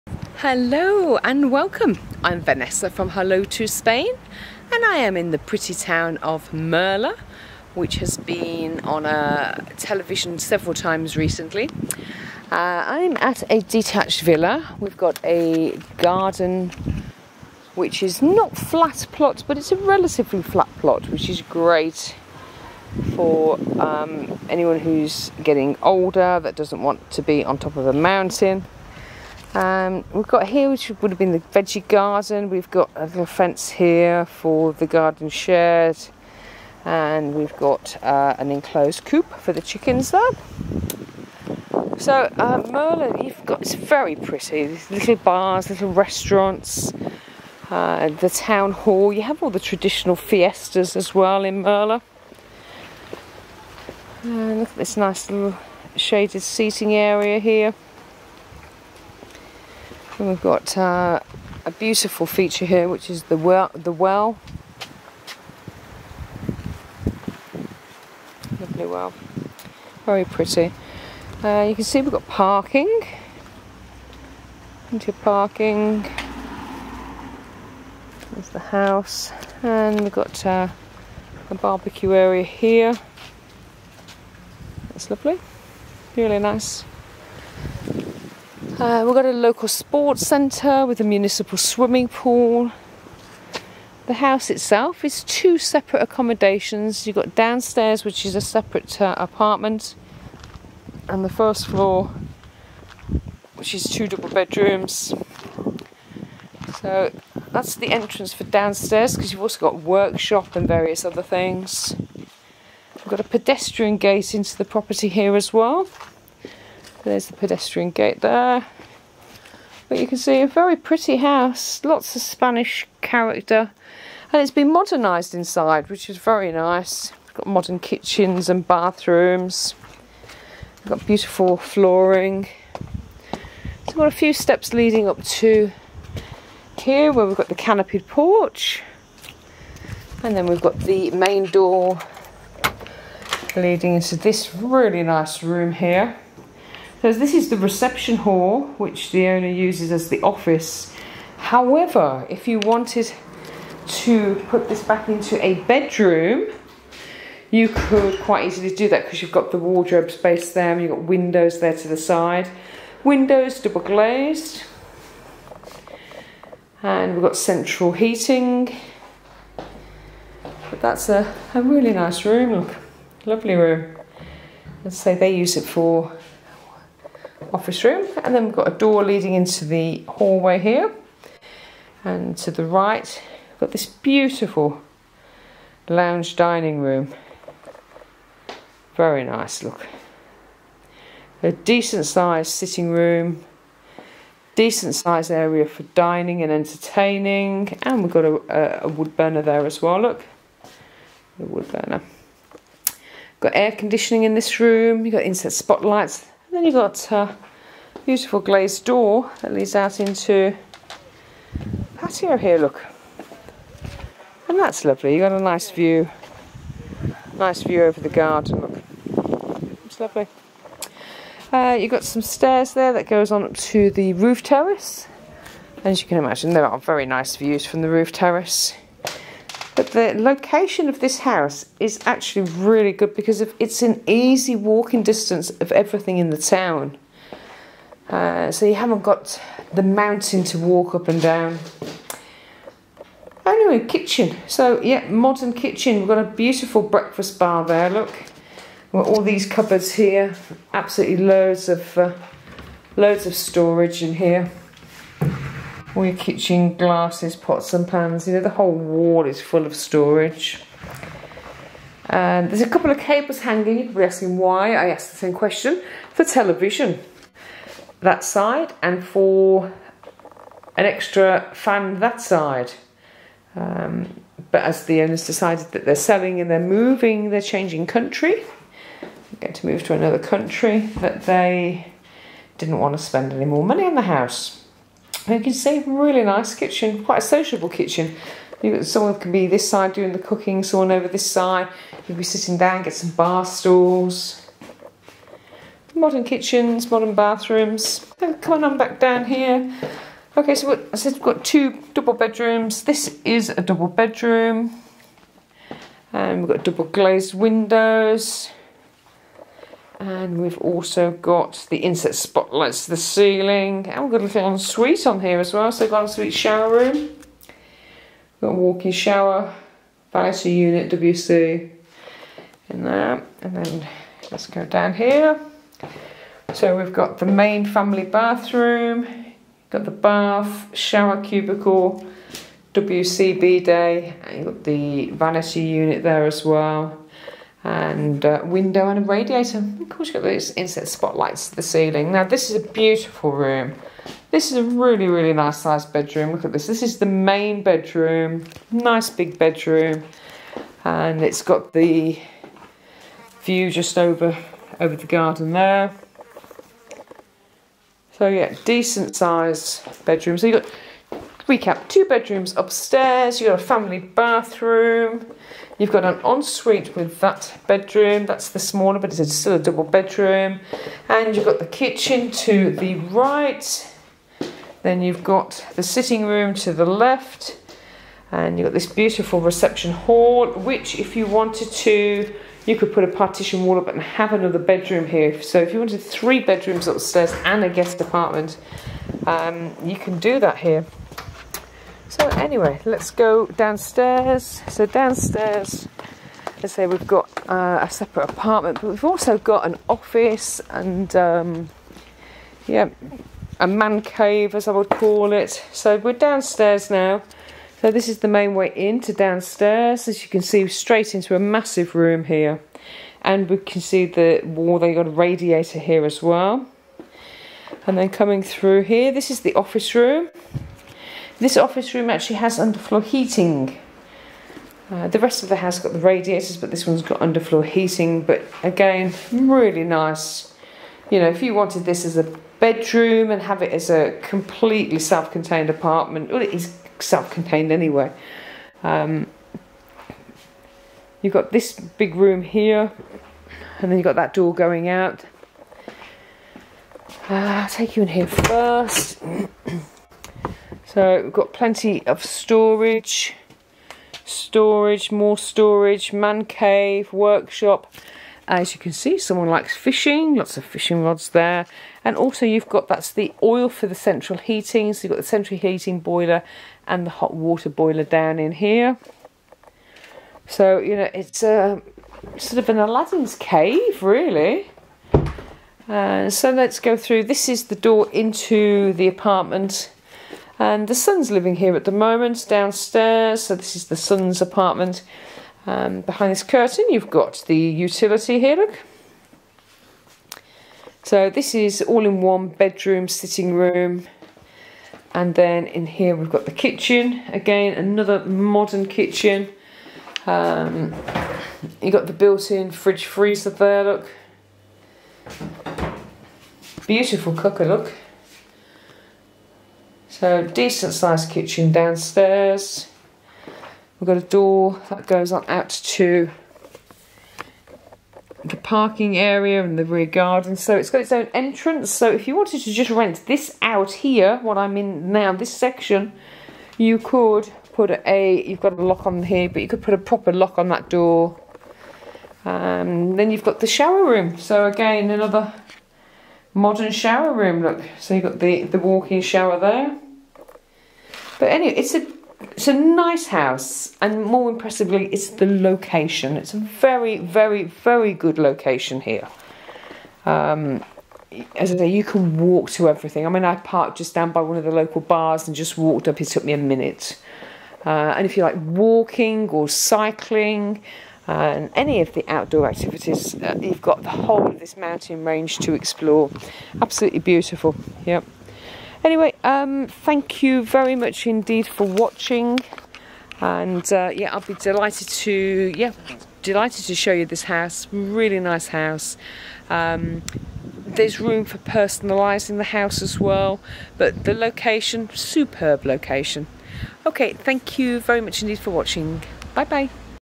SOLD SIMILAR PROPERTIES REQUIRED
Murla, a pretty town in the Jalon Valley. This detached villa is in walking distance to the main town and is situated in a relatively flat plot. The villa is two separate accommodations with the main living on the first floor and the ground floor with a guest apartment. Double glazing, central heating upstairs and downstairs, air conditioning.
The approach has a pedestrian entrance and a vehicular entrance. Off road parking for many vehicles with outside lighting and water.
The first floor is accessed via an external staircase with a canopied porch over the main door leading int a lovely reception.
Reception hall is a spacious room, currently used as an office with windows and lighting. If you wanted 3 bedrooms upstairs this would be easily separated to make an additional bedroom.
Lounge dining room with a wood burner, patio door leading to a terrace, air conditioning unit, window and lighting.
Kitchen with a range of base and wall units, sink, free standing cooker with extractor over, fridge freezer, breakfast bar and a window.
Bedroom 1 is a very spacious main bedroom with a window and ceiling lighting and fan.
Bedroom 2 is a double bedroom with a window, lighting and en suite.
En suite with a walk in shower W.C, vanity unit and lighting.
Family bathroom with a bath and a separate shower cubicle, W.C, vanity unit, window, bidet and lighting.
Ground floor accommodation: WATCH THE VIDEO AS IT IS VERY SPACIOUS WITH SEVERAL ROOMS.
Guest apartment with separate entrance leading into a studio lounge, dining room, bedroom, separate modern kitchen with free standing cooker, window, base and wall units, sink, window and lighting.
Ensuite shower room with a walk in shower, vanity unit, W.C, window and lighting.
Utility room with space and plumbing for washing machine, linen cupboard and shelving.
Store rooms/workshops the main room has a separate entrance from the parking area, window open doorway to the office room.
Office room with a window an underfloor heating.
Additional store room/workshop is another very useful space, storage, workshop with lighting.
The gardens are low maintenance with spacious terraces, BBQ area, feature well for mountain water and at the bottom of the garden a chicken coup. Plot 650m² approximately.
This villa is in a prime location, very little work required, modern kitchens and bathrooms, double glazing, central heating and a guest apartment.